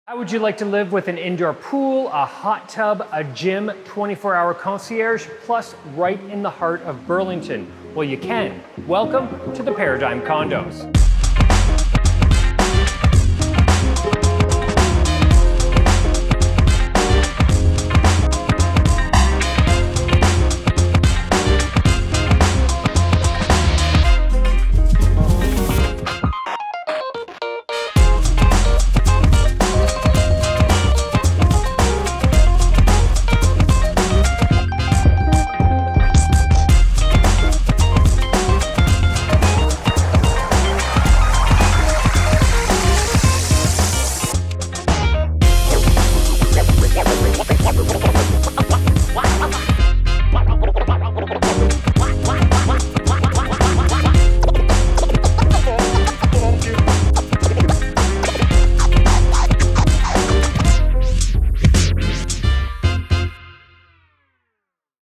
705-2081 Fairview Street - Burlington
Welcome home to this bright and open two-bedroom, two-bathroom condo in the prestigious paradigm building.

This 820-square-foot condo is one of the biggest two-bedroom layouts in the complex. Carpet-free living, south-facing views and good size kitchen.

Some of the best amenities in Burlington can be found in the paradigm: Pool, hot tub, gym, steam room, party room, sky lounge and 24 hours security. One parking, one locker. Steps to Burlington GO train and Walmart. Just a couple minutes away from Burlington’s waterfront and Highway access.